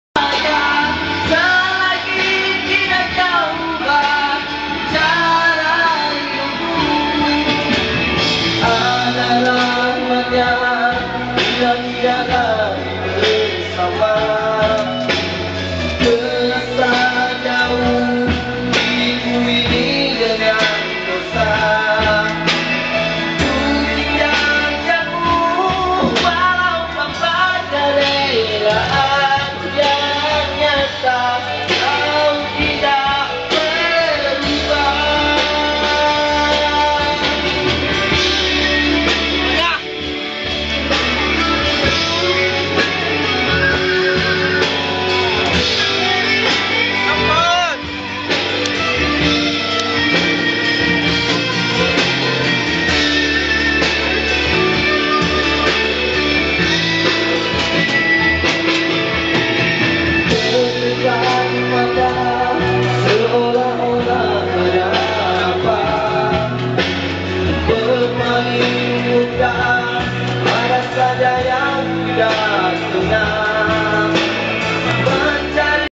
A Singer At The Wedding
a young guy singing at his sister's wedding. this video was taken at Merlimau, Melaka, Malaysia. normal situation in every wedding event, visitor will sing a song to entertain the bride.